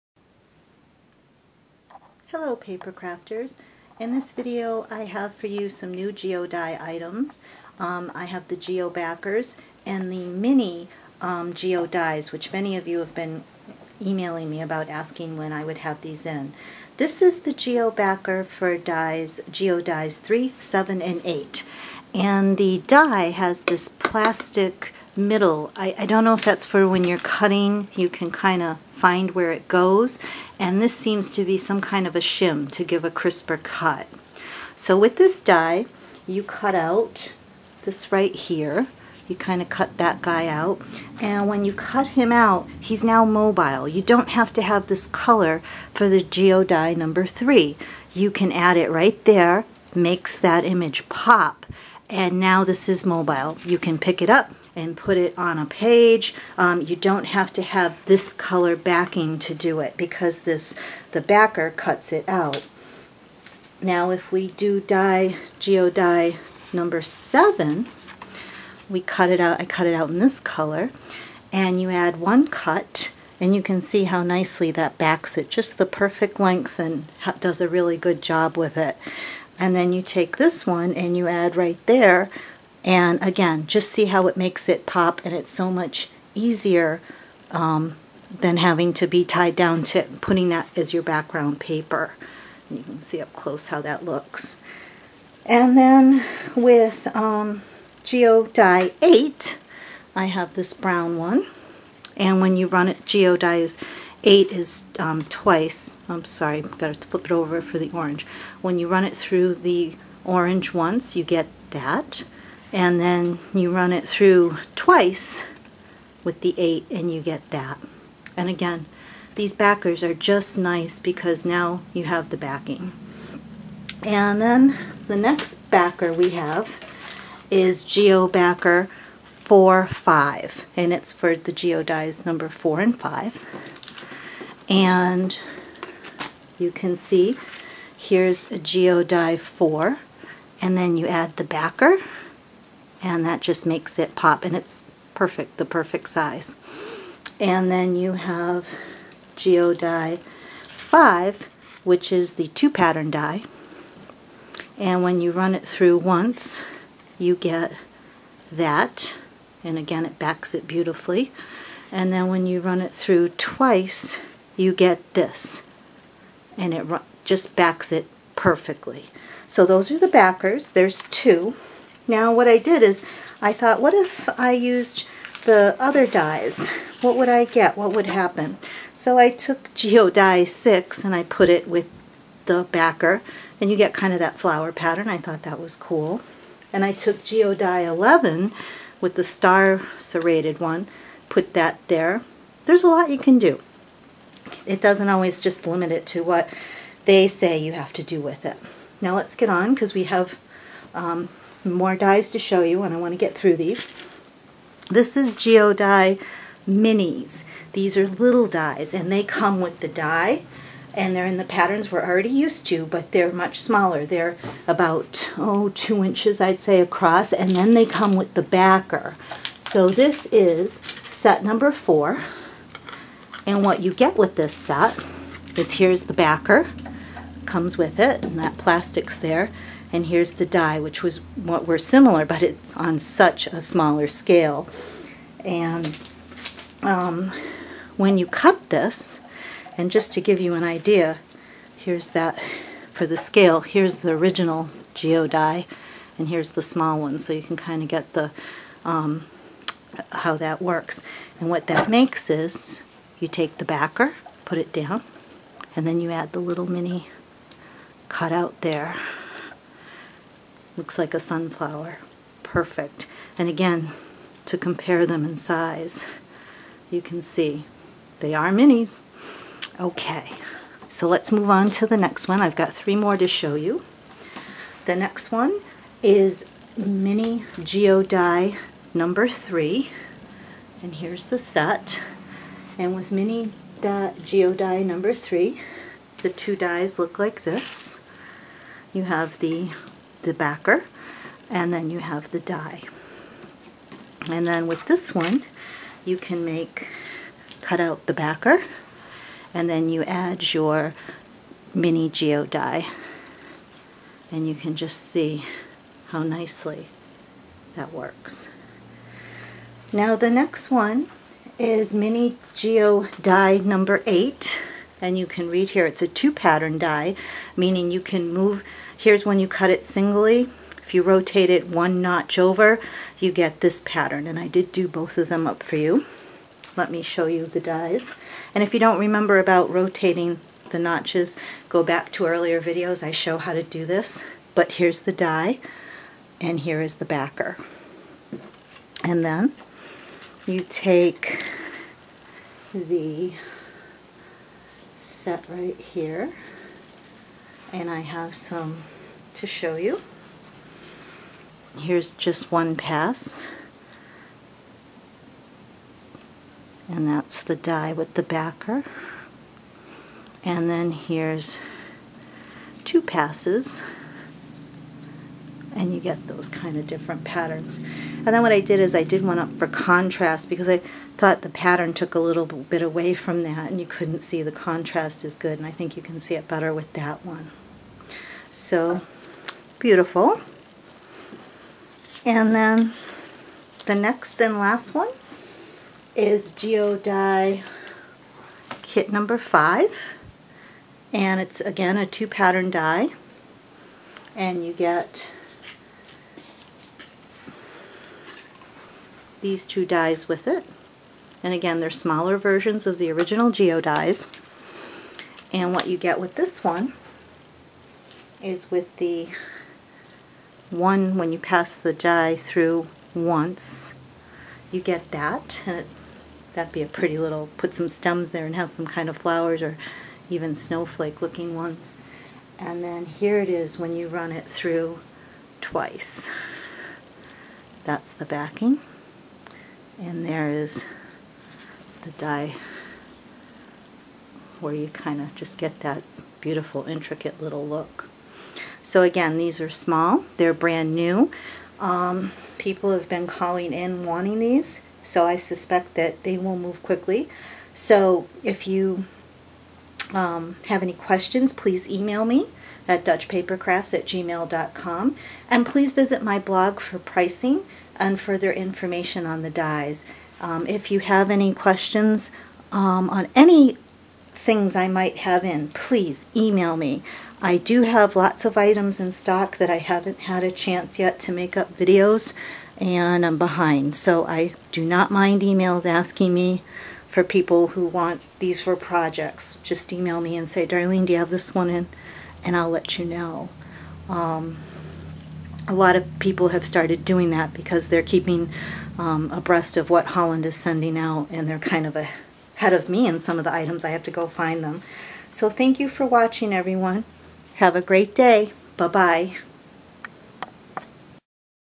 Geo Backers and Mini Geo Dies
In this video I show the new Geo Backers for Geo Dies 4/5 and 3/7/8 and the Mini Geo Dies 3,4,5 and 8 that I have for sale on my blog: dutchpapercrafts.blogspot.com.  Thanks for viewing.